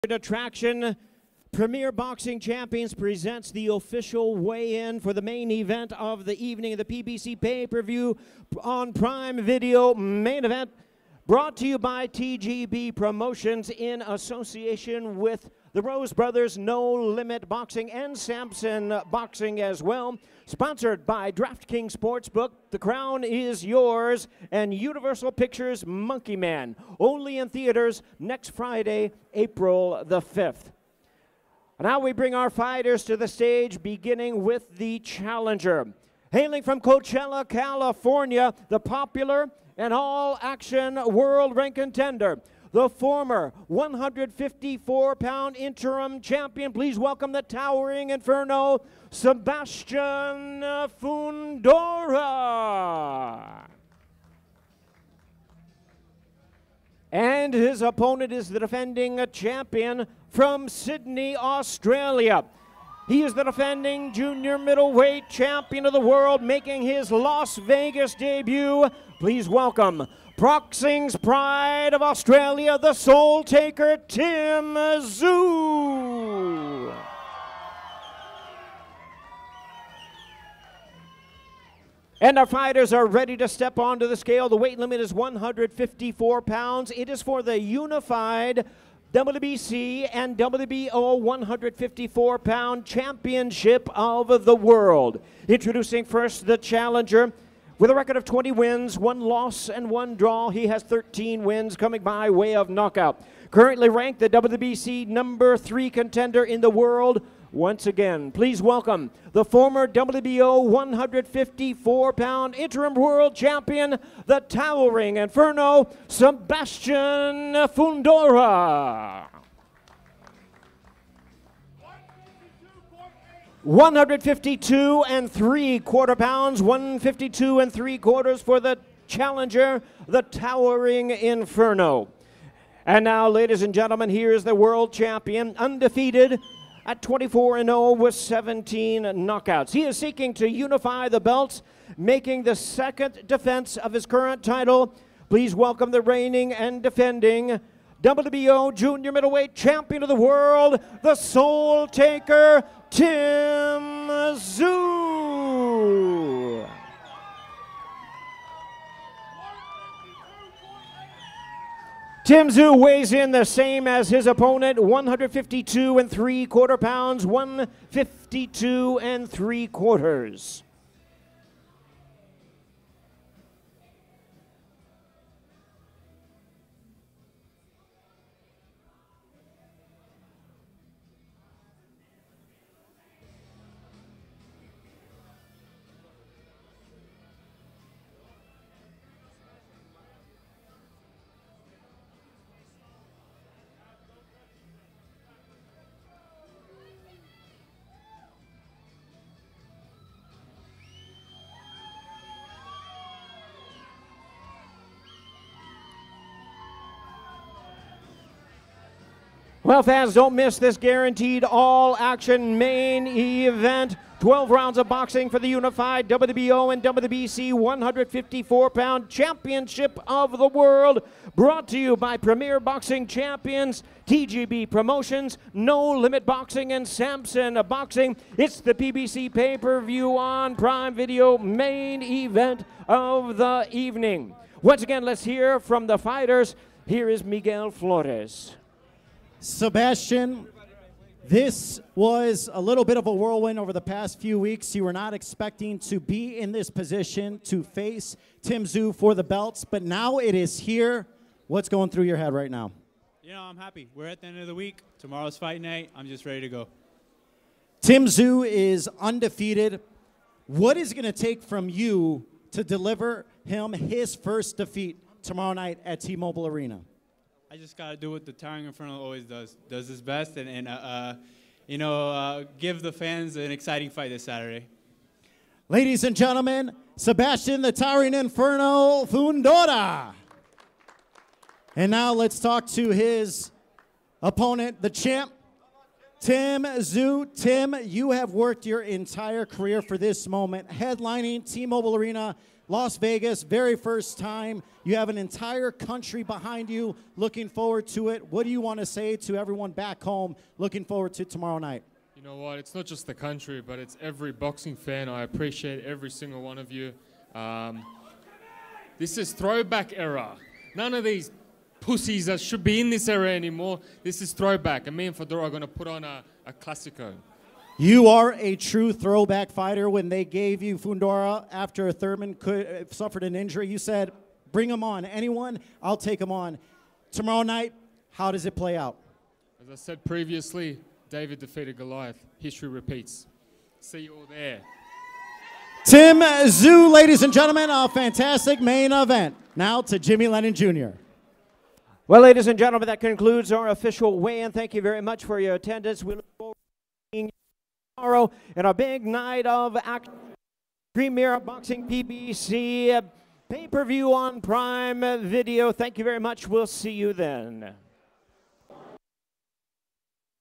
Tim Tszyu vs Sebastian Fundora • FULL WEIGH IN & FACE OFF Video
Tim Tsyzu and Sebastian Fundora both weigh in for their main event on Amazon Prime on PBC as the two fighters make weight and face off one last time ahead of their fight!